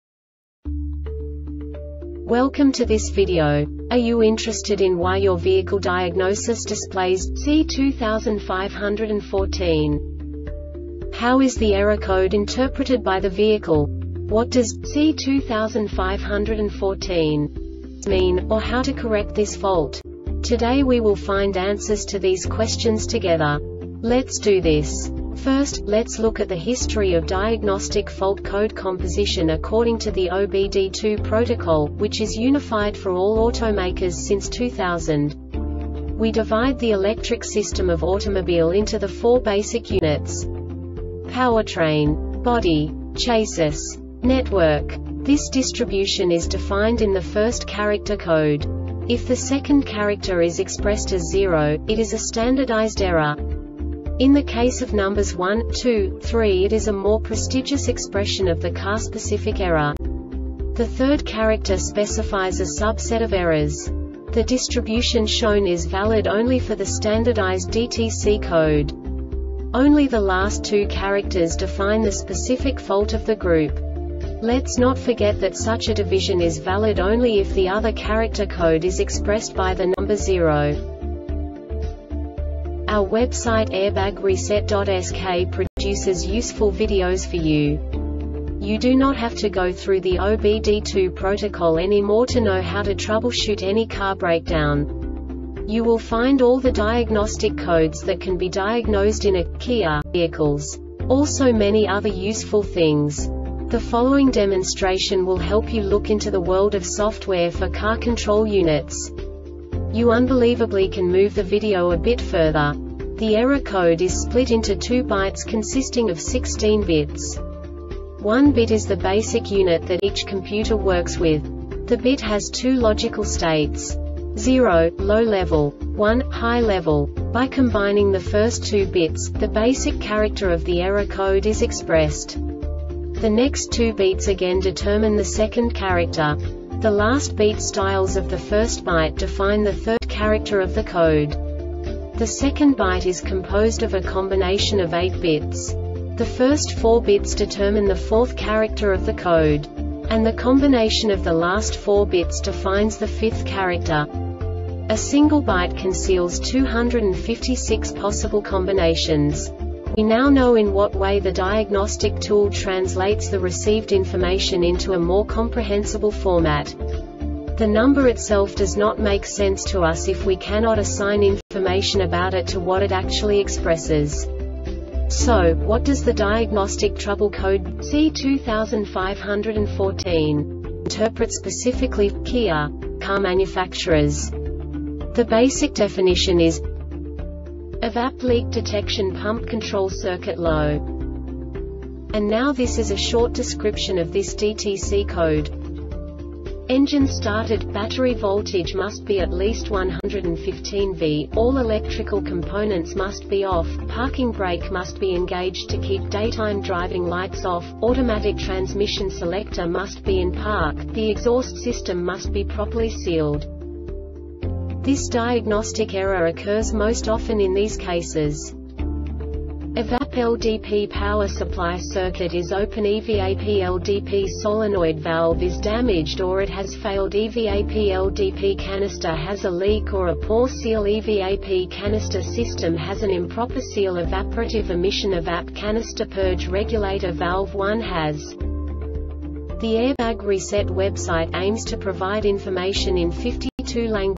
DTC KIA C2514 Short Explanation
Contents:
0:21 Basic DTC analysis according to OBD2 protocol standard.
1:48 Insight into programming
2:58 A diagnostically understandable description of the Diagnostic Trouble Code
3:05 Basic error definition

Make:
Kia
Code:
C2514
Definition:
EVAP Leak Detection Pump Control Circuit Low
Description:
Engine started,battery voltage must be at least 11.5v,all electrical components must be off,parking brake must be engaged (to keep daytime driving lights off),automatic transmission selector must be in park,the exhaust system must be properly sealed
Cause:
EVAP LDP power supply circuit is openEVAP LDP solenoid valve is damaged or it has failedEVAP LDP canister has a leak or a poor sealEVAP canister system has an improper sealEvaporative Emission (EVAP) canister purge regulator valve 1 has